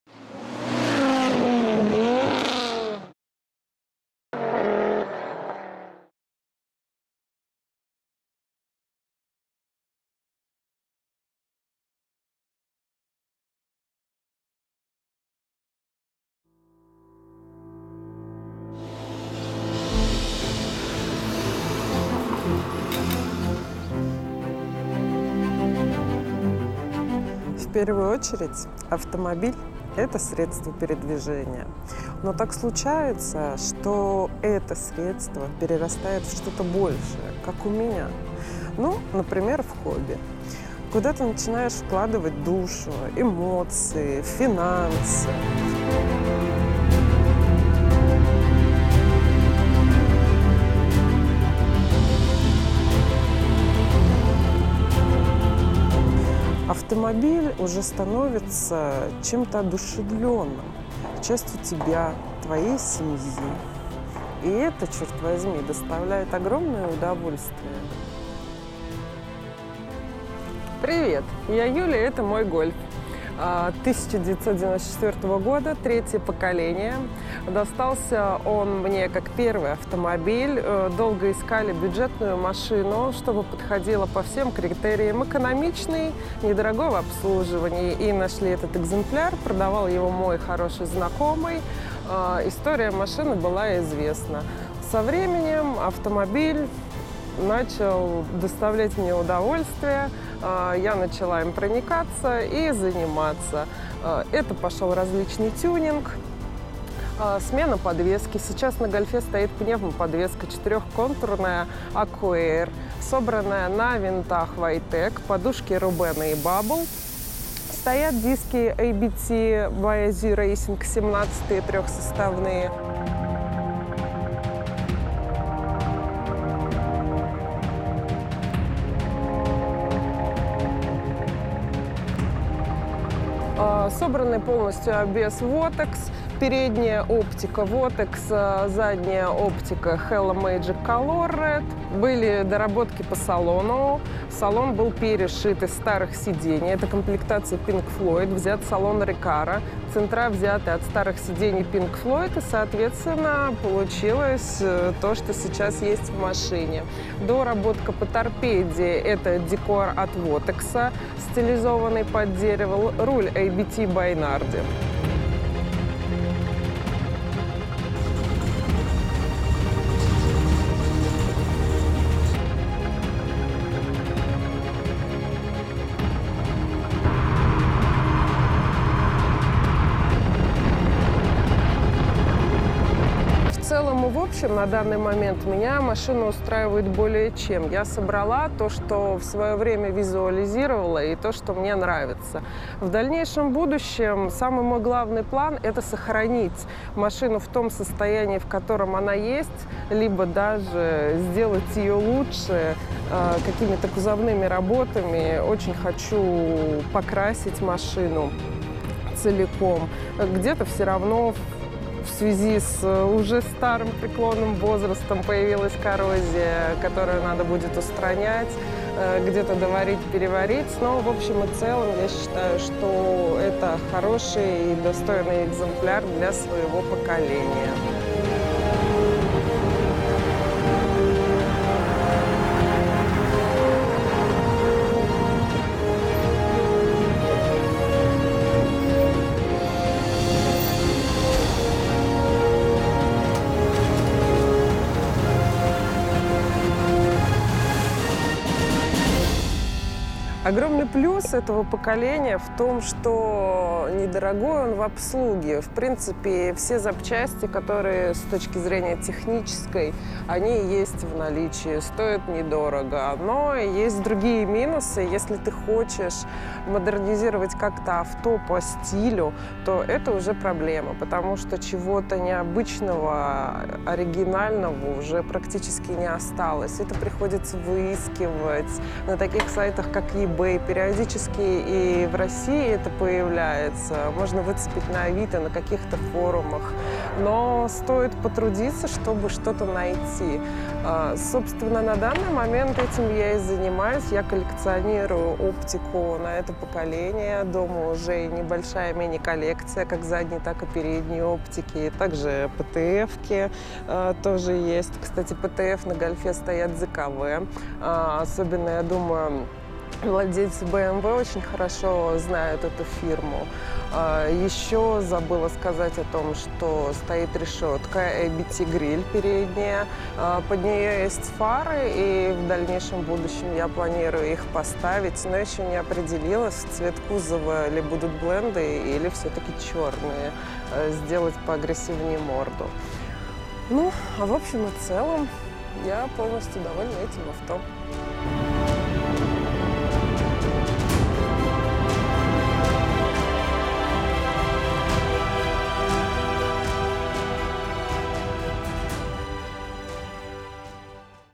MK3 AIR & STANCE - ДЕВУШКИ ТОЖЕ МОГУТ // АВТОБИОГРАФИЯ | SB MEDIA
Вторую серию мы сняли про VW GOLF MK3 на пневмо, который построила девушка Юля @julijke. Изначально это было обыкновенный гольф 3го поколения на автомате с мотором 1.8, со временем превратившийся в один из самых заметных и красивых стэнс гольфов в Санкт-Петербурге. Теперь и у вас есть возможность познакомиться с этим тандемом поближе!
My Gear :
Lens Sigma 30mm F1.4 Contemporary DC DN Lens for Sony E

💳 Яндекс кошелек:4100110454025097